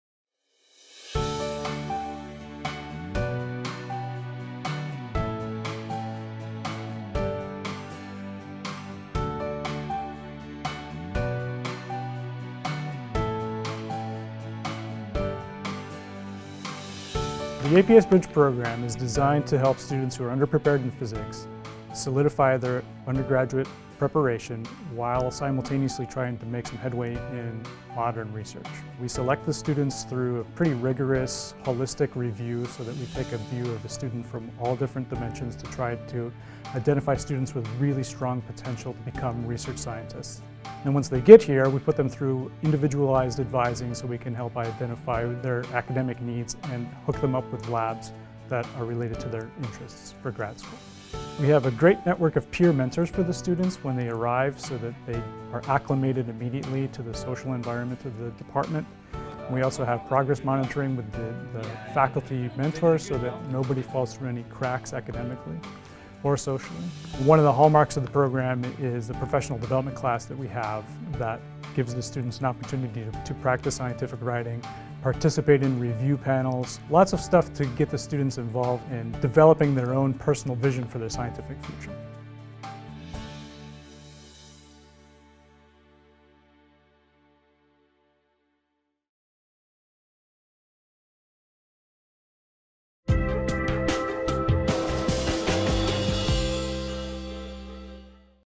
APS Bridge Program - New Opportunities
The APS Bridge Program gives students great opportunities they otherwise might not have in physics and to increase the number of physics PhDs to under-represented minority students -- APS TV went to one of the Bridge Program sites at the University of South Florida to find out how.